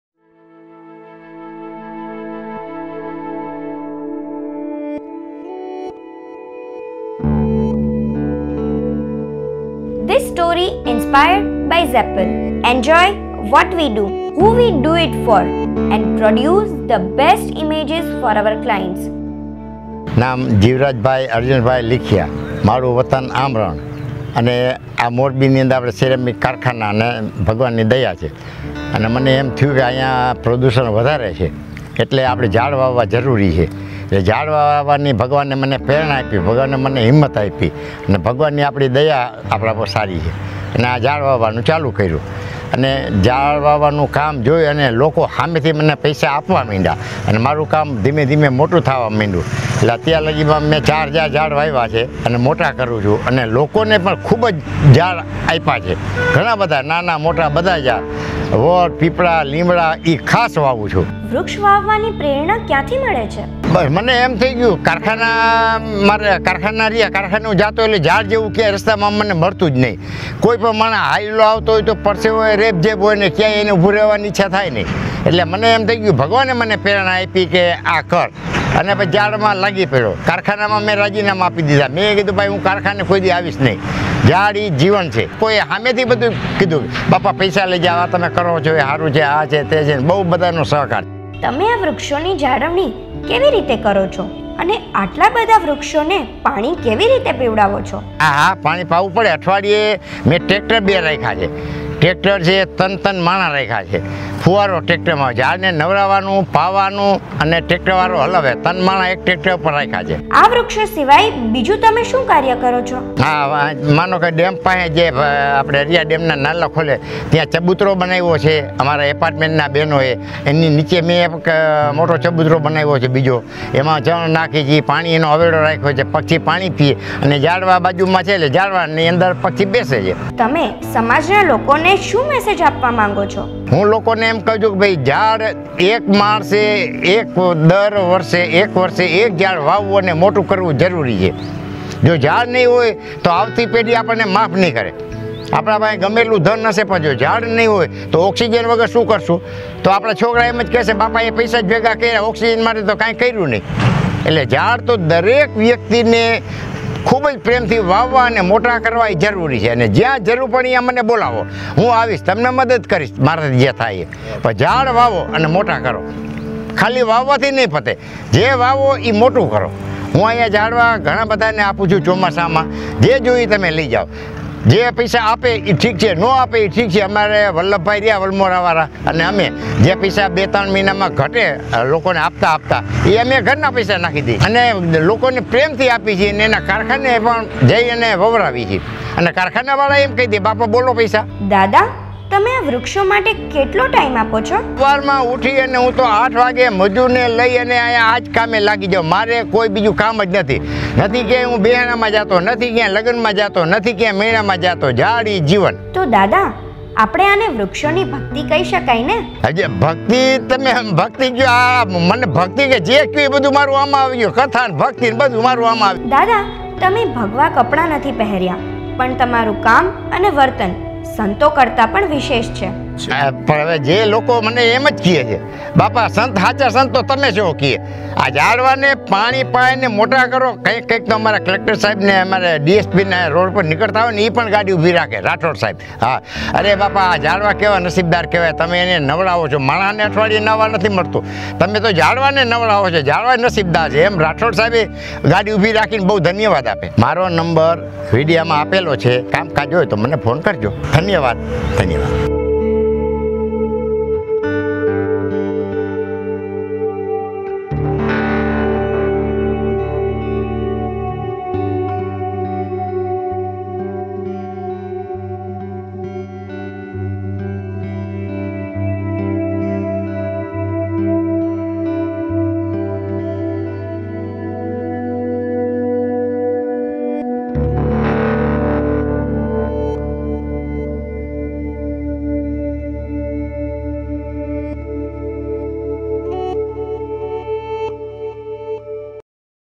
great inside story Tree planting | every person should plantation one tree in their life
The Planet Earth category contains subcategories called "Uncharted", "That's Amazing", "Aquatic World", "On the Brink", and "Into the Outdoors".

Voice over: Vanshika Gokani
Content: D.K.D Siva
Supported: ZAPPL Team &  Avnish Gadhvi
Special thanks: V.D Bala Navrang Natural club